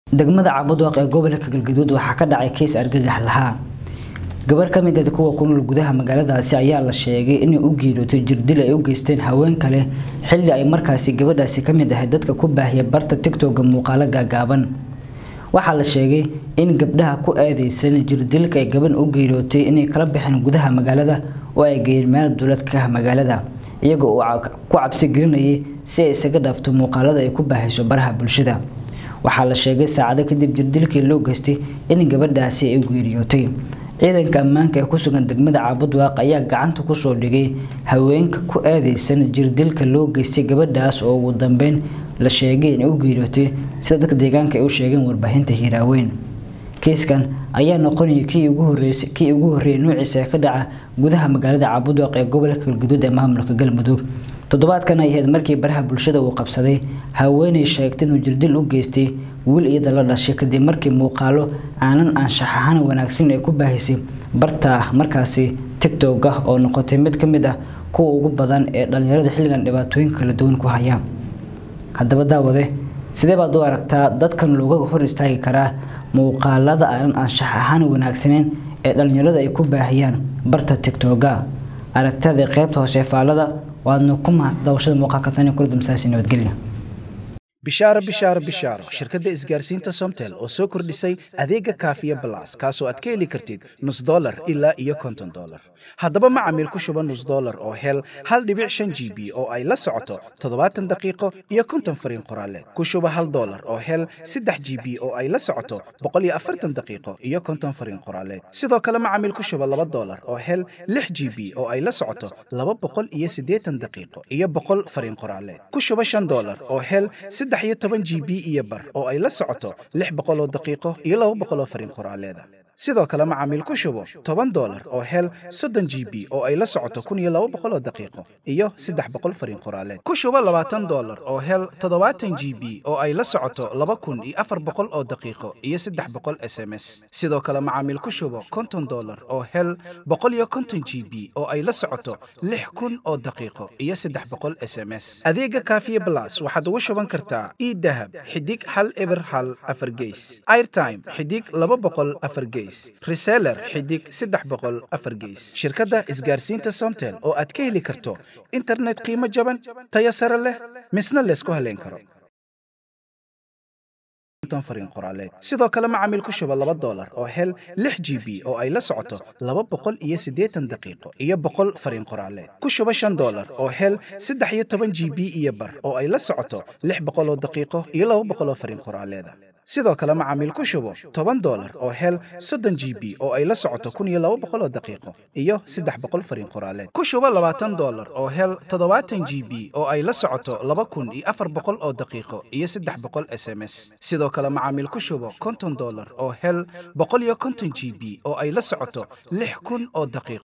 Gabar u geeriyootay jirdil loo geystay ka dib markii ay barta TIK-TOK ku baahisay muuqaalo.
Gabar dhalinyaro ah oo u geeriyootay jirdil loo geystay ka dib markii ay barta TIK-TOK ku baahisay muuqaalo.